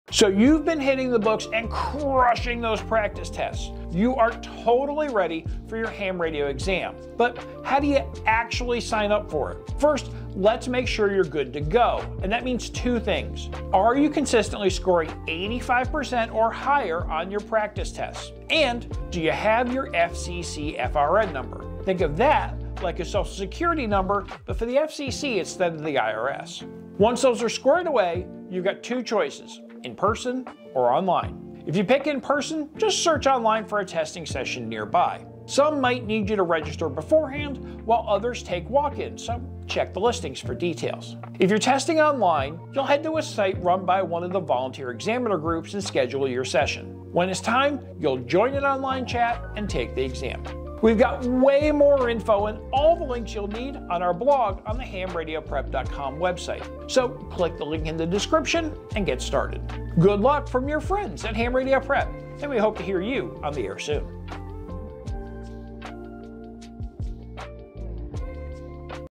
Ready for Your Ham Radio Exam? Here's How!
Crushing practice tests? Time to schedule that ham radio exam! Two things: 85%+ scores and your FCC FRN. Choose in-person or online, find a session, and ace it! Ready to jump in? for more